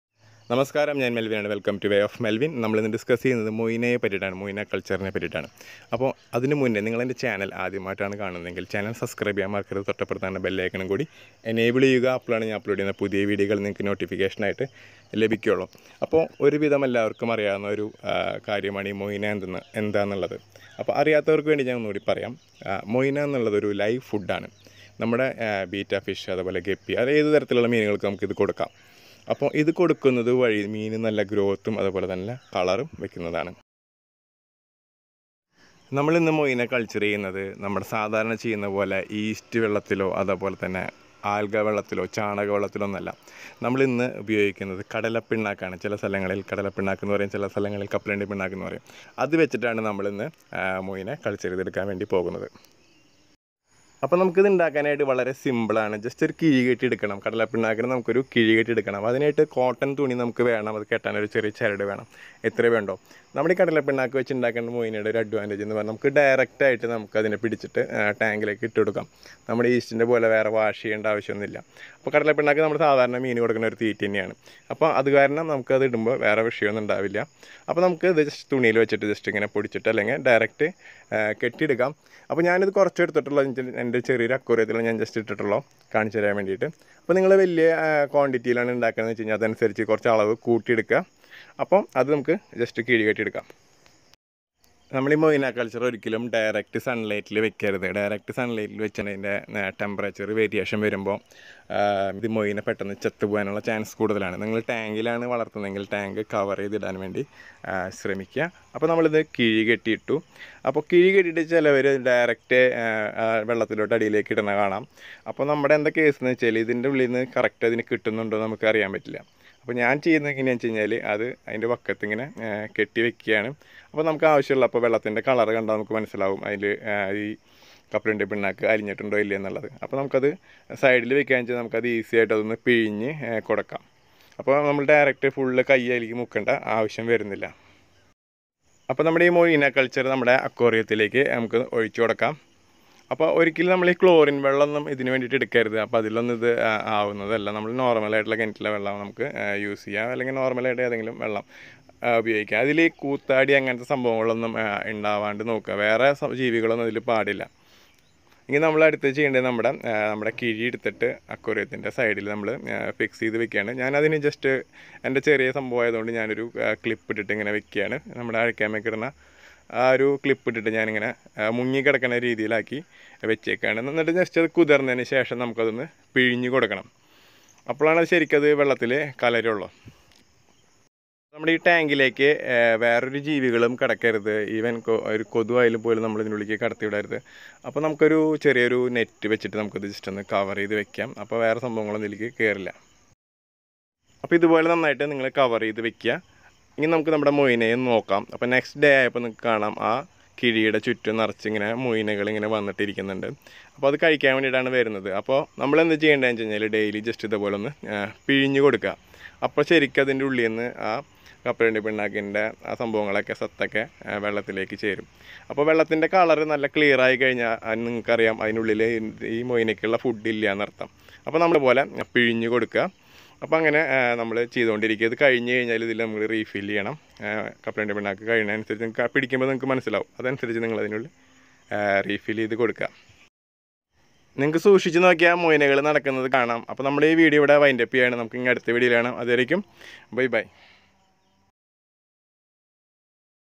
യീസ്റ്റും ആൽഗ വെള്ളവും ഇല്ലാതെ മോയിന എങ്ങനെ കൾച്ചർ ചെയ്യാം,how culture moina in malayalam,
യീസ്റ്റും ആൽഗ വെള്ളവും ചാണകവെള്ളവും ഇല്ലാതെ മോയിന കൾച്ചർ എങ്ങനെ ചെയ്യാം, how to culture moina in malayalam,